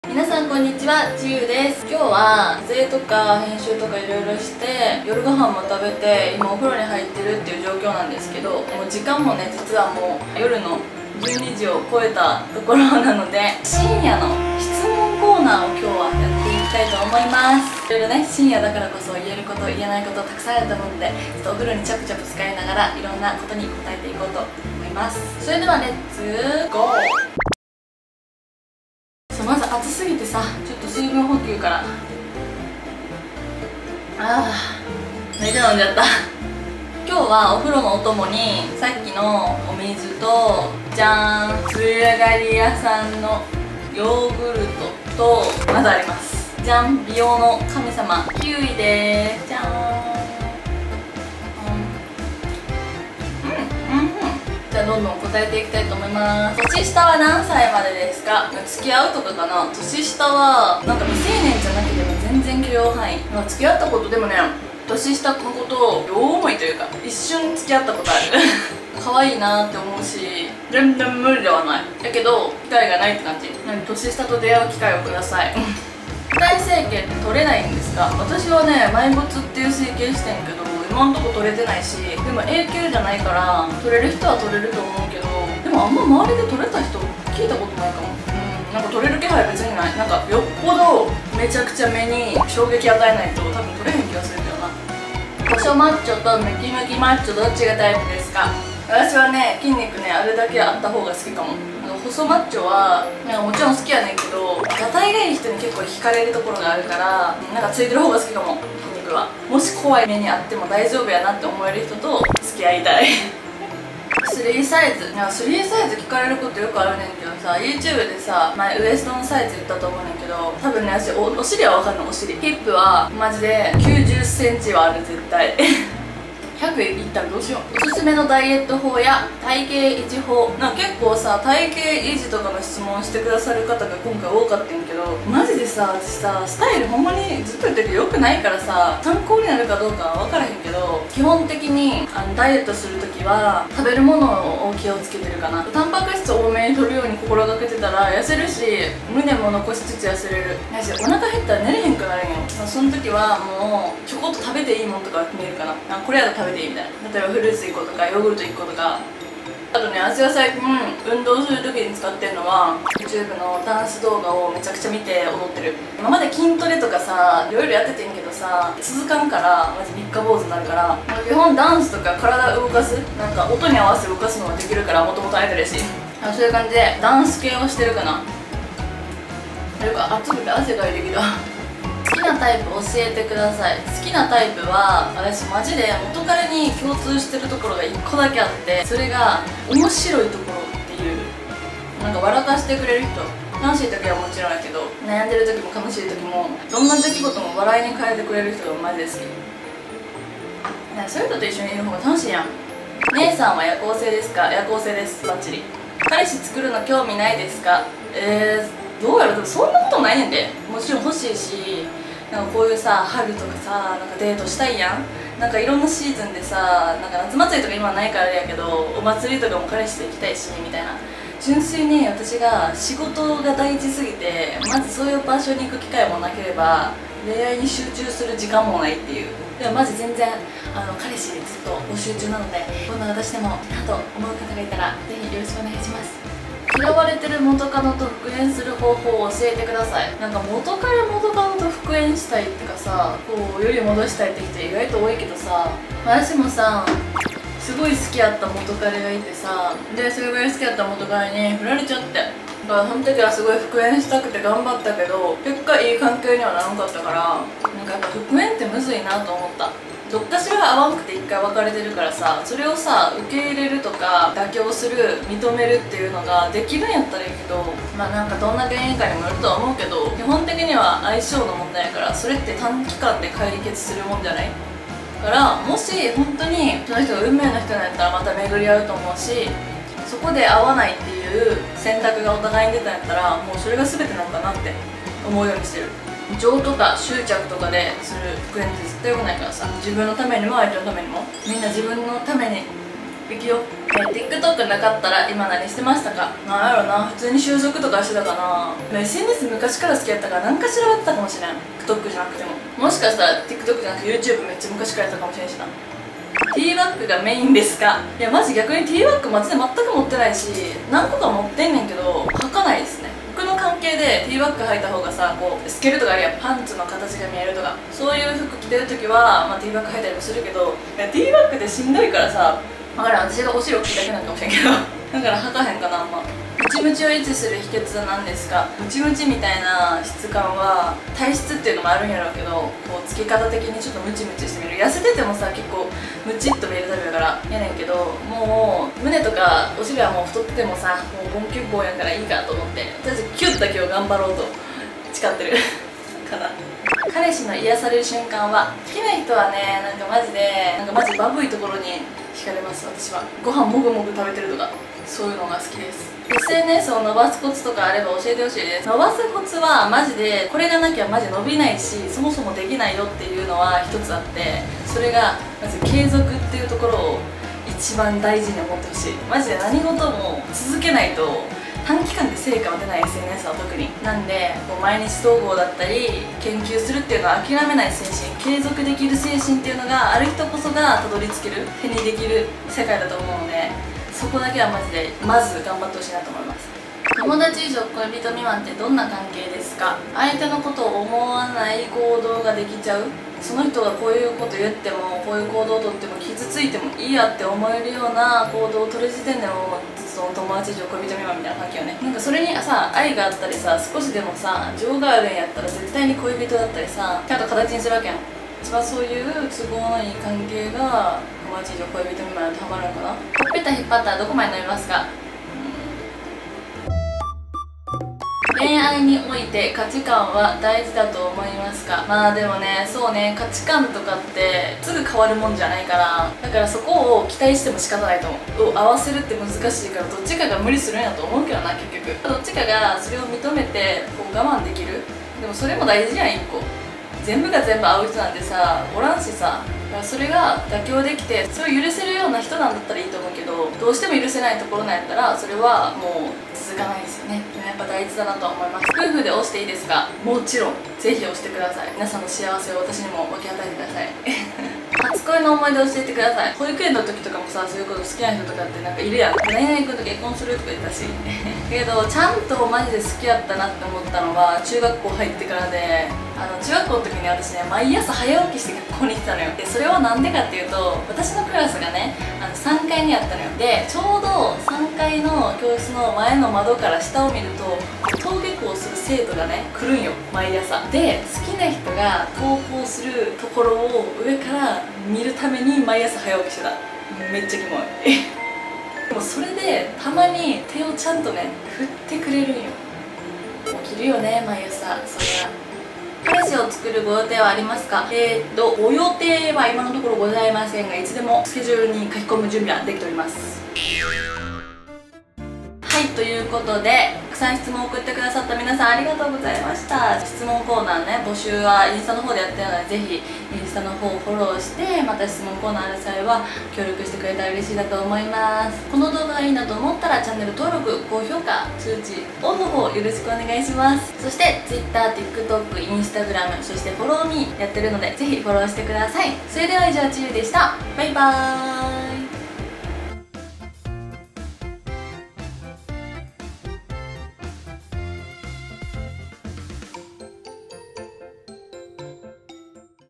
【お風呂】深夜テンションで色々暴露しちゃったw
やほやほ〜！
今日も動画みてくれてありがとう🎶

 今日は お風呂でちゃぽちゃぽしながら
たくさんの質問に答えてみたよっ❕🛁🌙

夜テンションっていいよね〜〜〜笑
ついついいろんなこと暴露してしまった、、

また次回の投稿もみてね！
ばいちゃ！^._.^♡